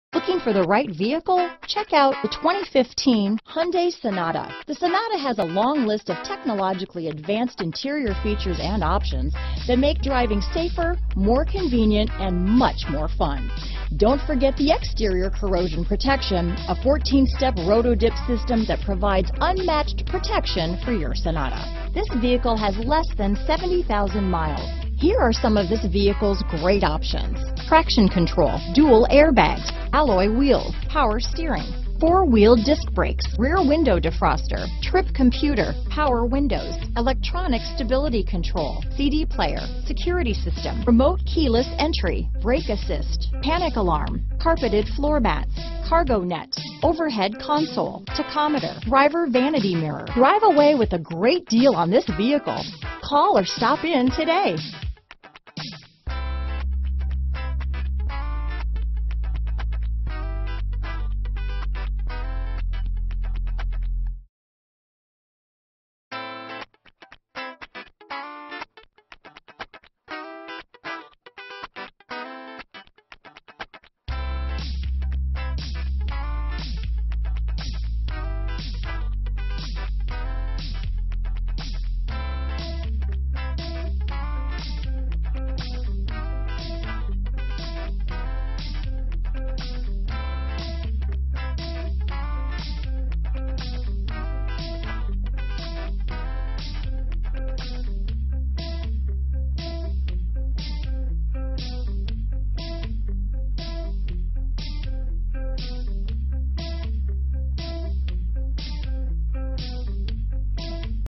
2015 Hyundai Sonata Schaumburg, Arlington Heights, Hoffman Estates, Des Plaines, Palatine, IL 92576A
Lakeside Blue Used 2015 Hyundai Sonata available in Schaumburg, Illinois at Schaumburg Honda. Servicing the Arlington Heights, Hoffman Estates, Des Plaines, Palatine, IL area.

Schaumburg Honda
750 E Golf Rd

Recent Arrival! Priced below KBB Fair Purchase Price! CARFAX One-Owner. 2015 Hyundai FWD Sonata ECO Lakeside Blue 7 Speed Dual Clutch DCT Clean CARFAX. Odometer is 7937 miles below market average!